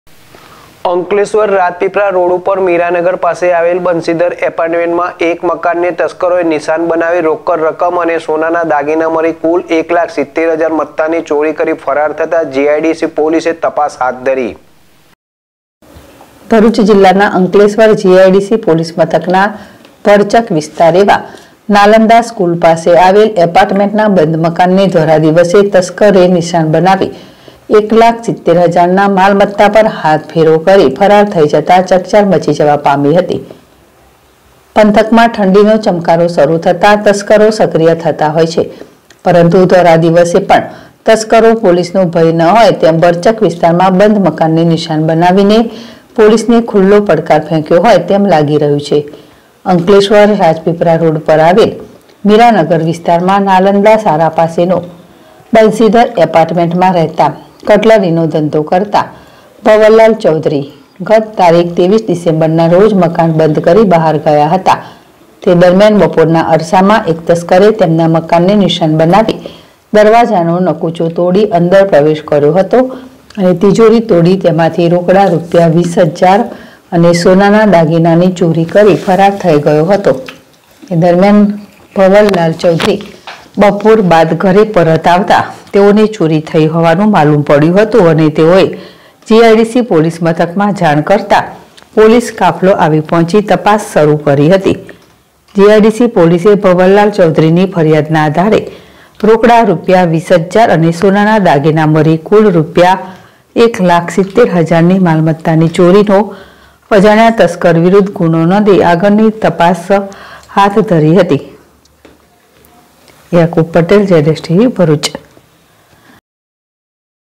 અંકલેશ્વર રાજપીપલા રોડ ઉપર મીરાનગર પાસે આવેલ બંસીધર એપાર્ટમેન્ટમાં એક બંધ મકાનને ધોળા
અંકલેશ્વર રાજપીપલા રોડ ઉપર મીરાનગર પાસે આવેલ બંસીધર એપાર્ટમેન્ટમાં એક બંધ મકાનને ધોળા દિવસે એક તસ્કરે નિશાન બનાવી